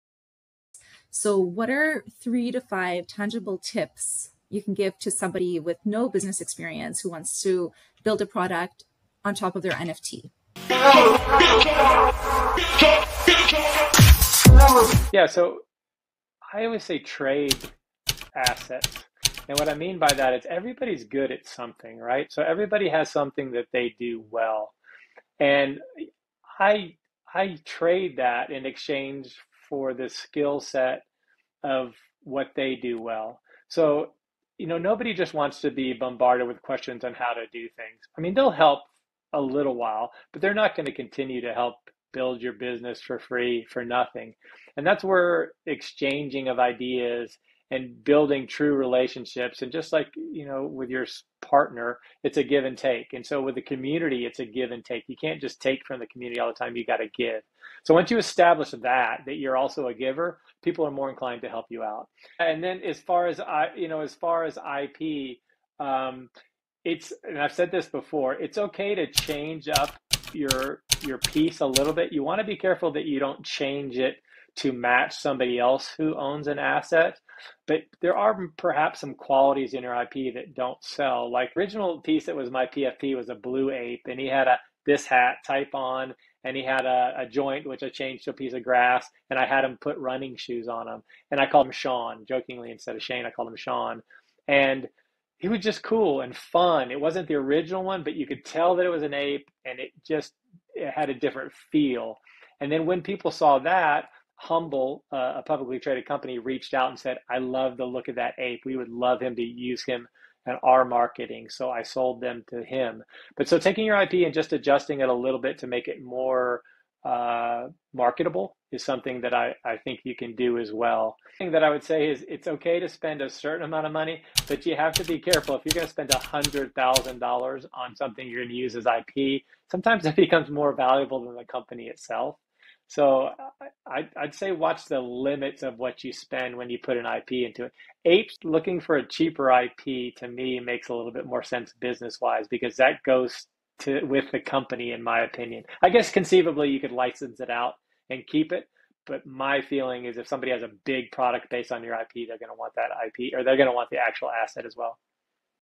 3 tips on building a product with your IP
Shane Cultra gives 3 tips on how to a build a product with your IP